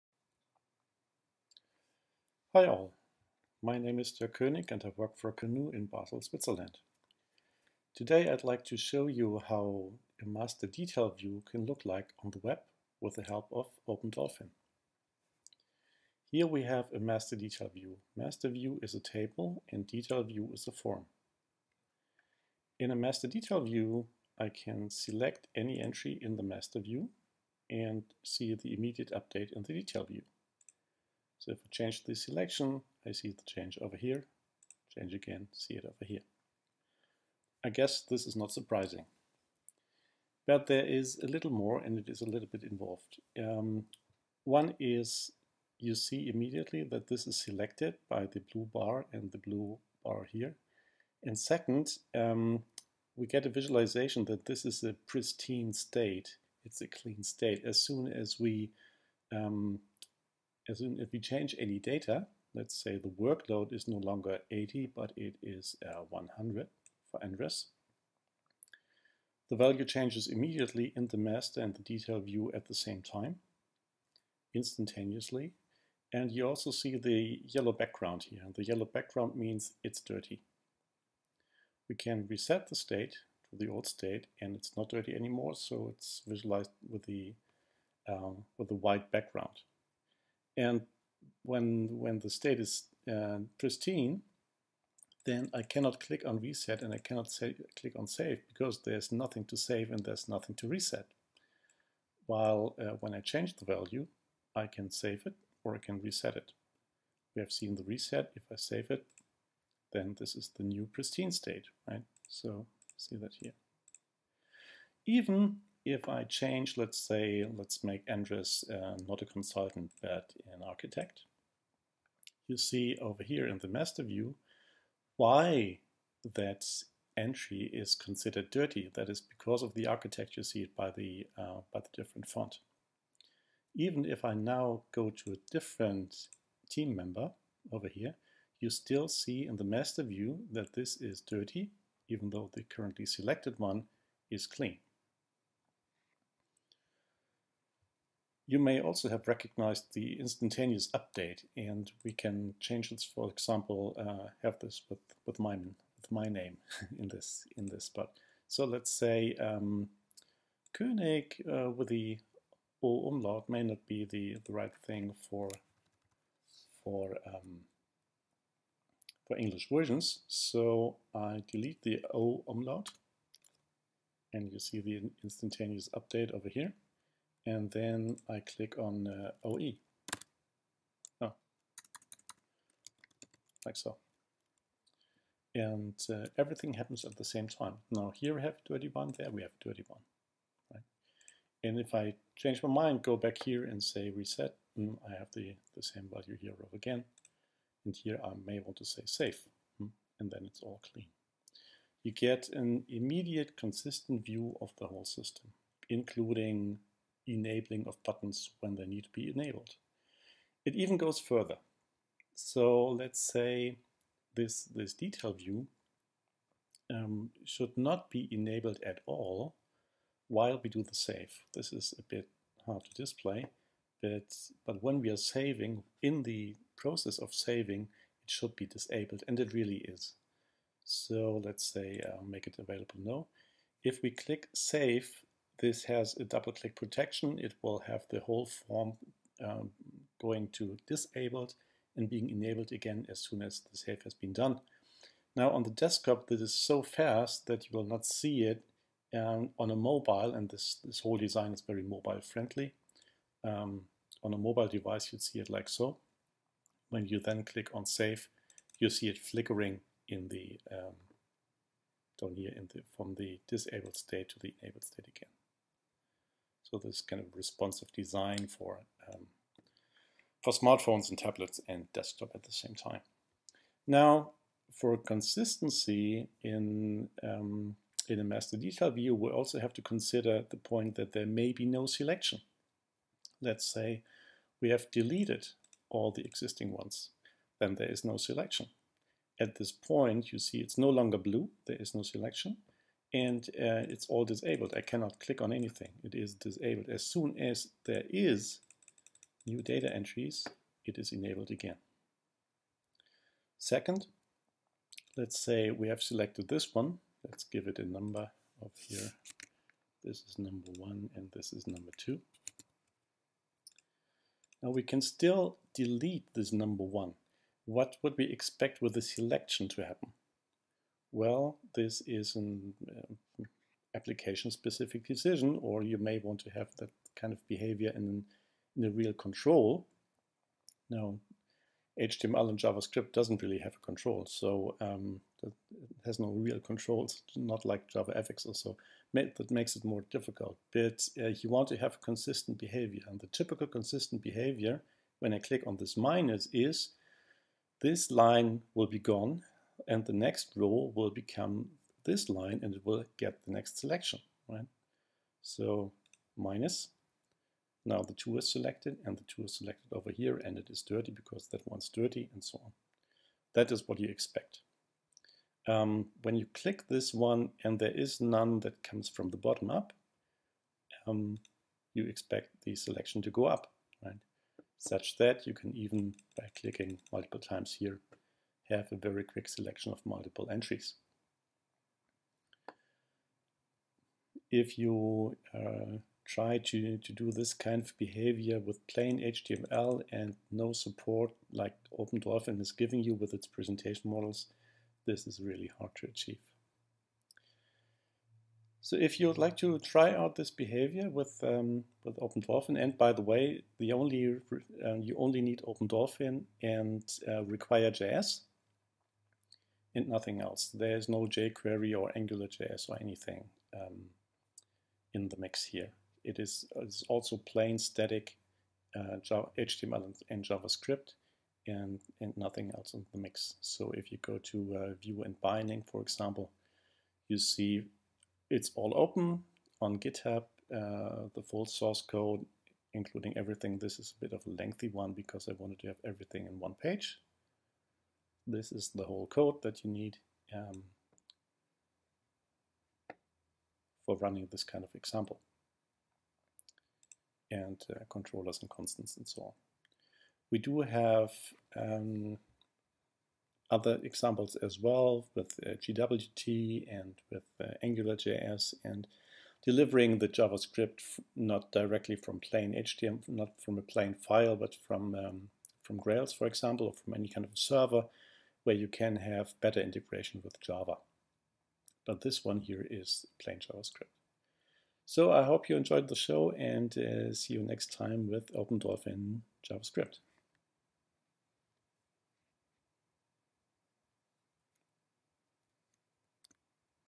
OpenDolphin.js master-detail demo
A master-detail view for the web with OpenDolphin.js featuring instantaneous updates, save, reset, consistent enabling/disabling of controls, visualization of selection and dirtyness, and double-click protection in a responsive design (using bootstrap).